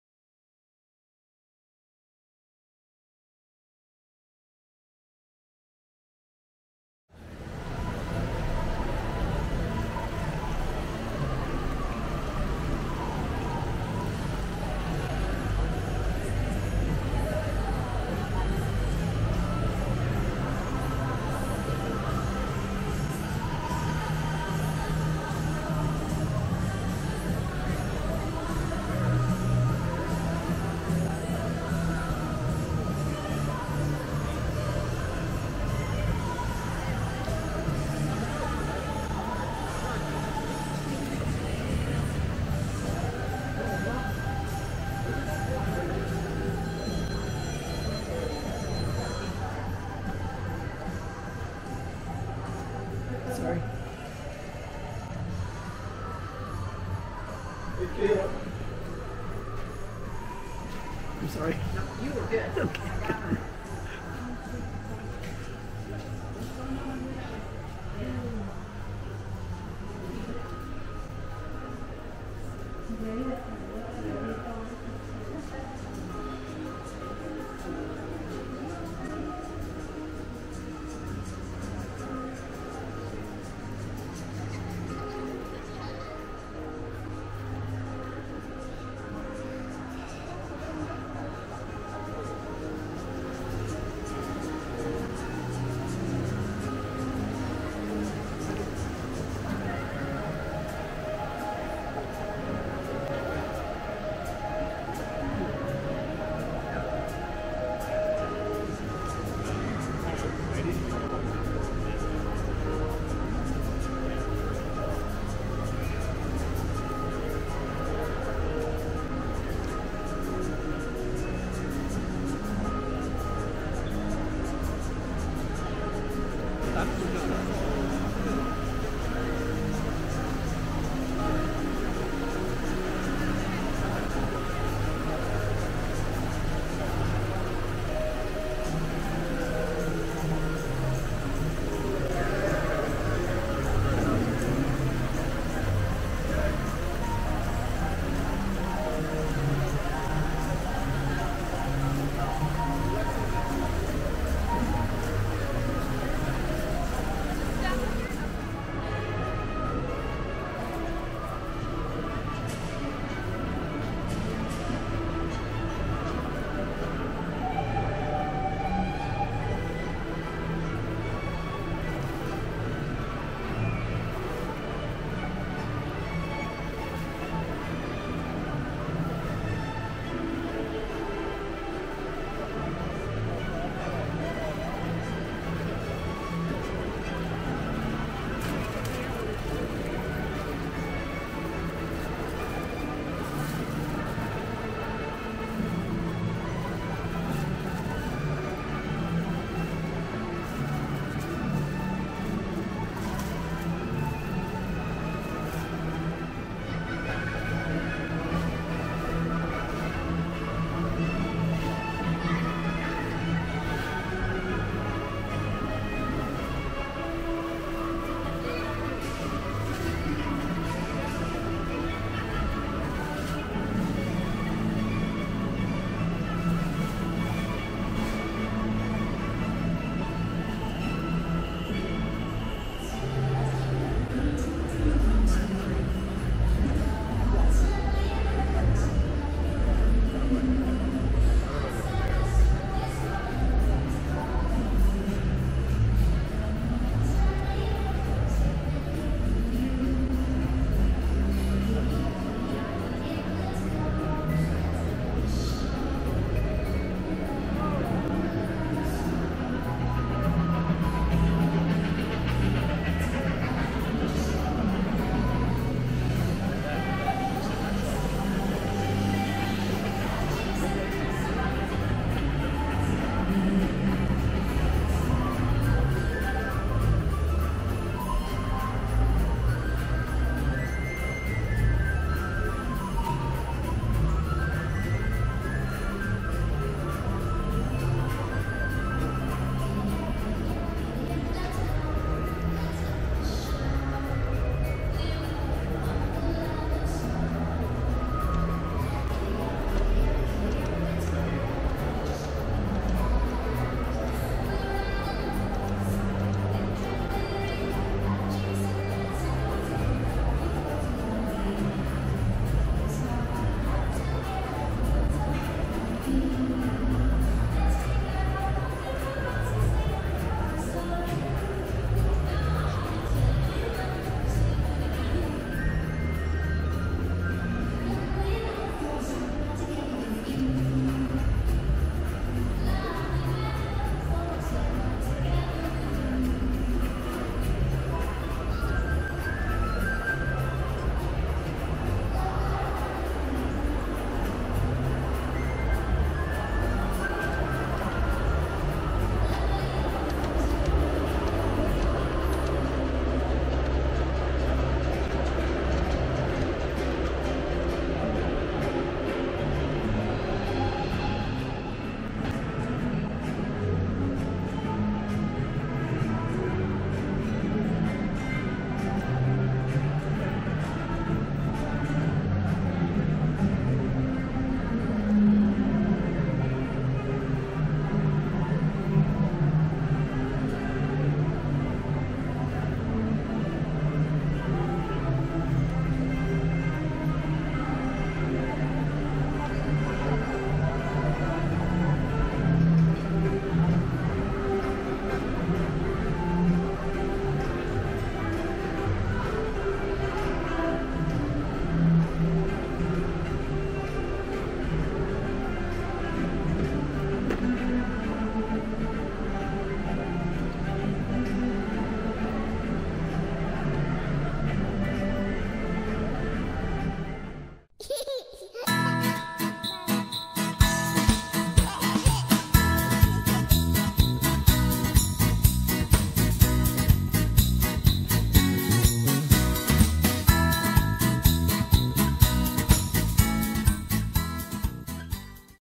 Bellagio Conservatory Christmas 2022 - MCAL Does Vegas
Christmas at the Bellagio Conservatory is always absolutely beautiful! Come check it out as we visit in the early morning, unfortunately while they were updated some of the displays, but still gorgeous!

Shot with the Osmo Pocket 2.